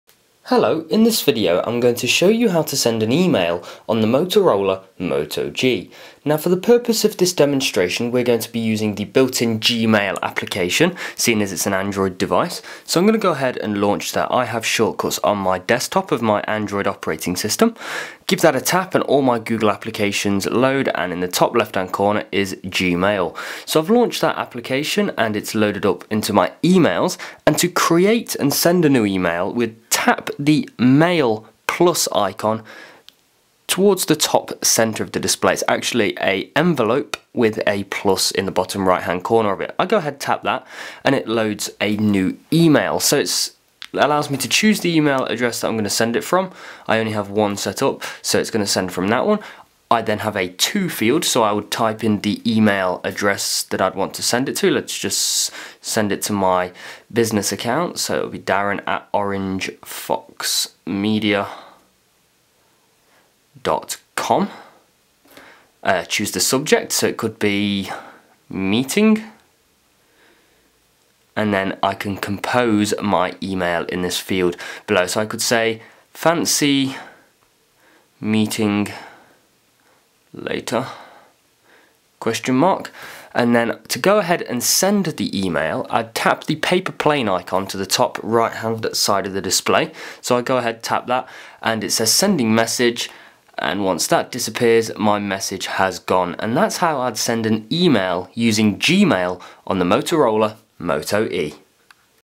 How To Send An Email - Motorola Moto E
A video how to, tutorial, guide on sending an email on the Motorola Moto E.